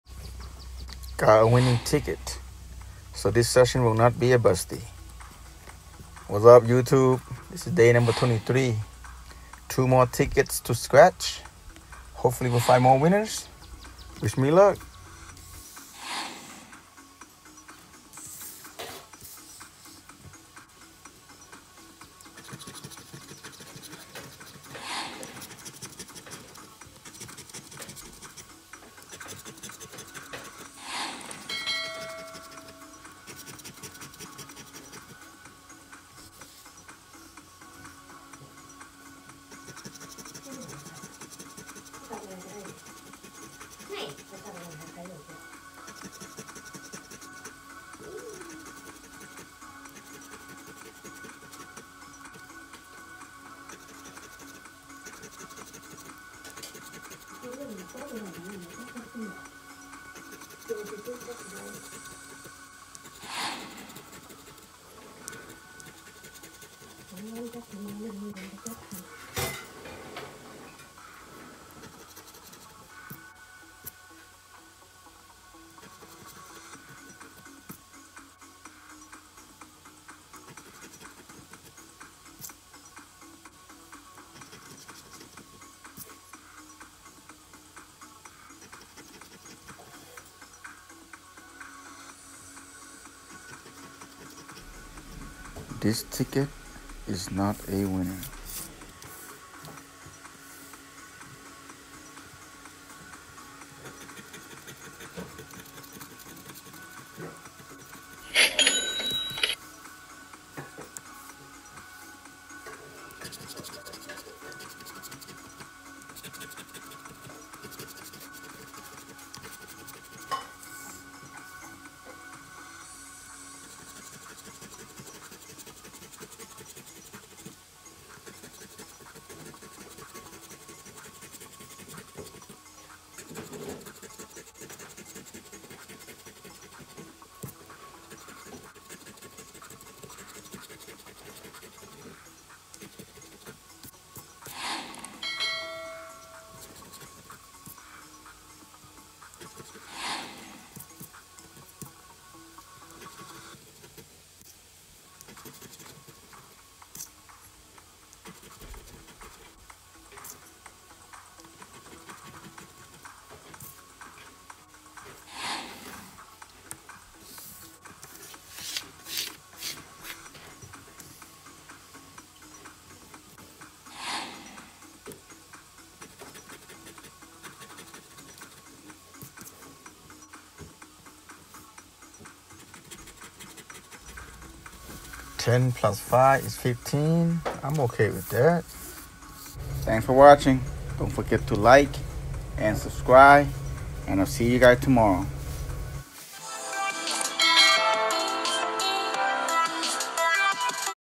30 Days Challenge | Day 23/30 | Cash Blast | Winner! Winner! No Busty on Day 23 💪💪👍😁
Hello everyone.

Iowa Lottery
Cash Blast
$10 Ticket

30 Days Challenge!
Month of June 2022
30 Days of Scratching
2 Tickets Per Day
60 Tickets in Total
$600 in Tickets

Day-to-Day Update!